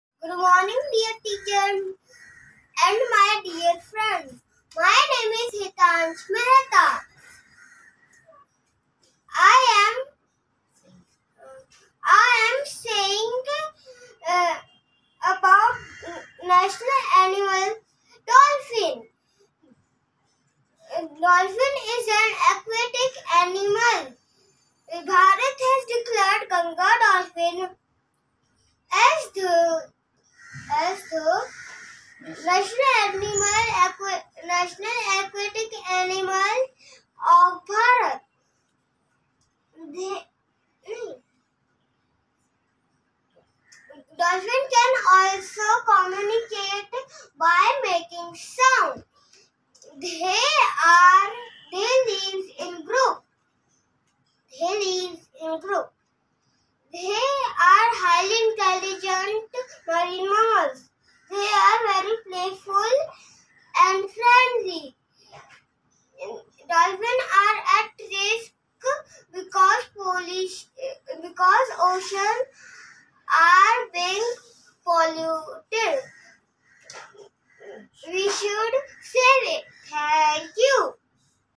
National Aquatic Animal Ganga Dolphin | Lines on Ganga Dolphin by Hetansh
The Ganges river dolphin is important because it is a reliable indicator of the health of the entire river ecosystem. The government of India declared it the National Aquatic Animal in 2009.
Be aware of Dolphins
Save them